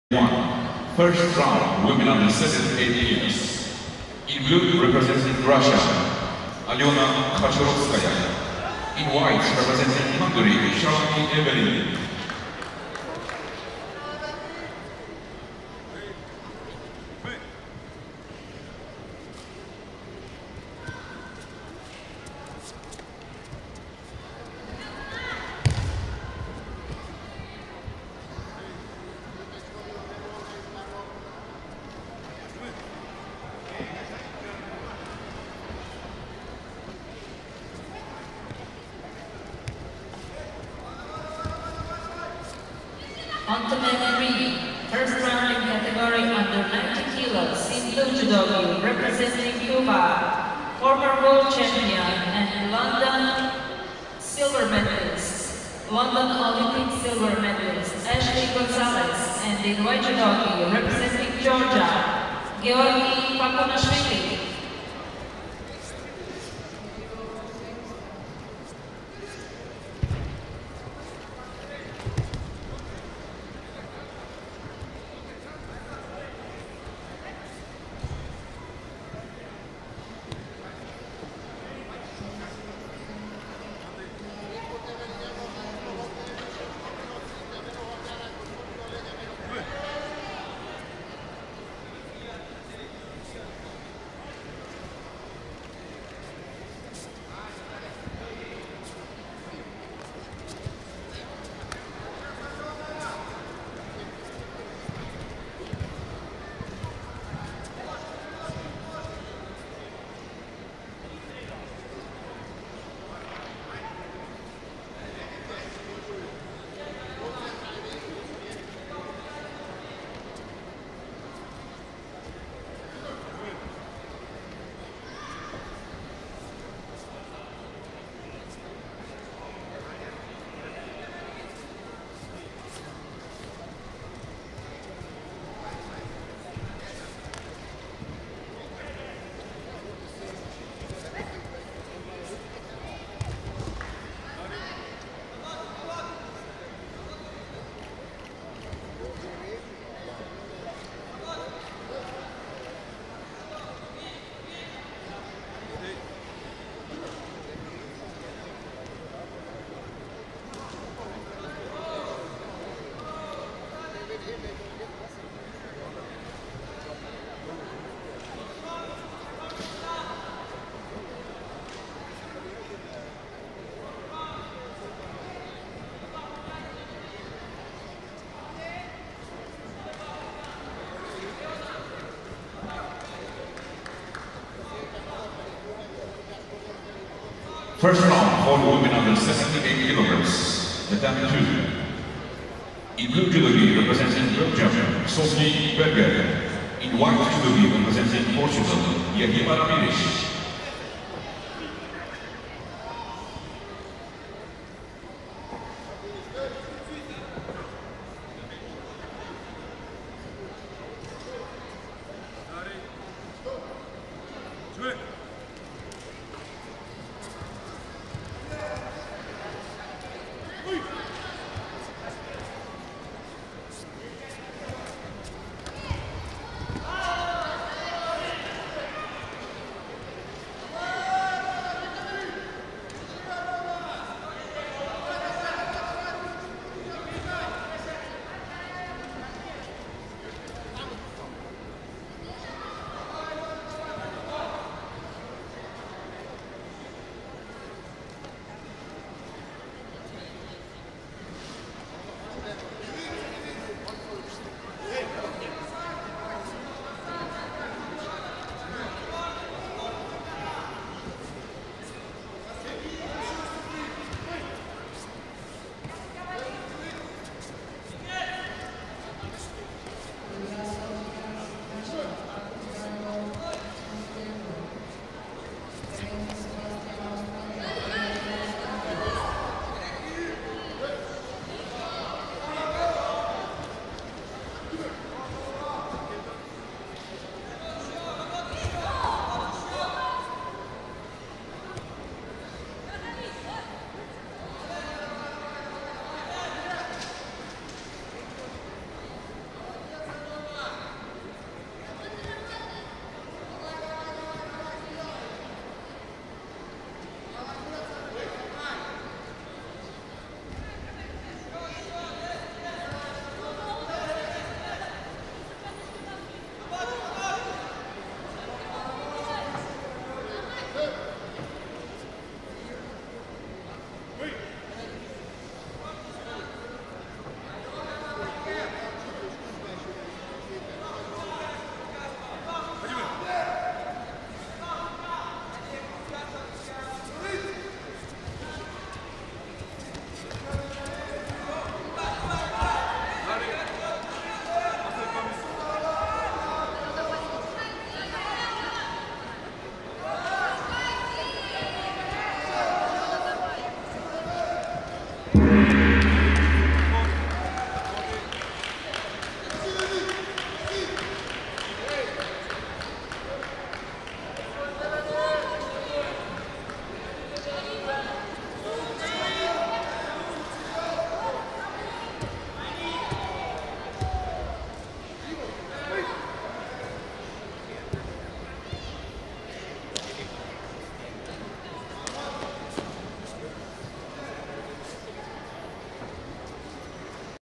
women judo osaekomi 287
Evelin SALÁNKI (HUN) - Alena KACHOROVSKAYA (RUS) U78, Grand Prix Zagreb 2018